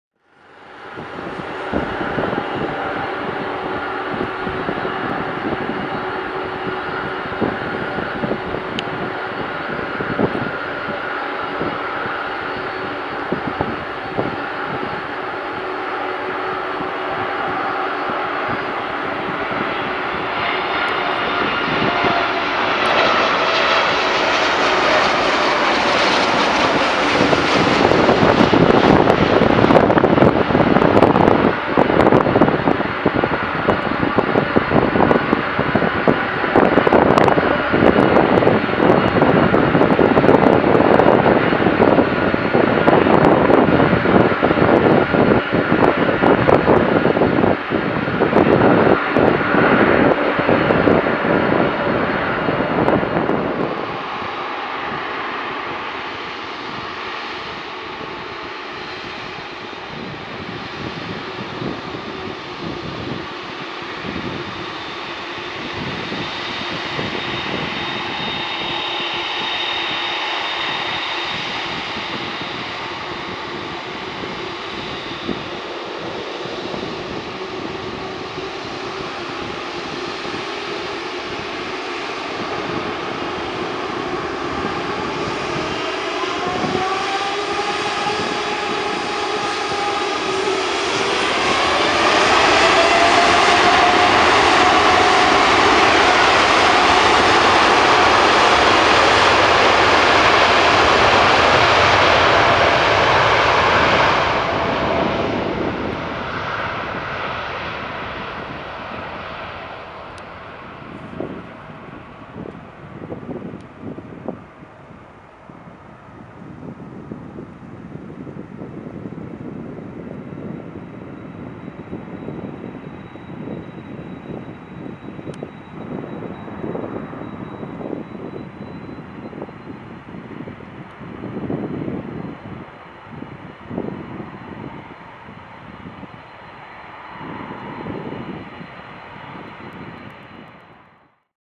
Cathay Pacific Cargo Boeing 747-8F [B-LJB] Arrival and Departure @ Anchorage Int'l (PANC)
Arrival info:
"Cathay" 91 from Hartsfield-Jackson Int'l (KATL)
Duration: 6 hours 49 minutes @ FL380

Departure info:
"Cathay" 91 to Hong Kong Int'l (VHHH / HKG)
Duration: 11 hours 56 minutes

Aircraft info:
Boeing 747-867F [B-LJB]
C/n: 39239
Engines: 4 x GEnx-2B67
Delivered: November 23rd 2011 - 2 years old

Date filmed: October 12th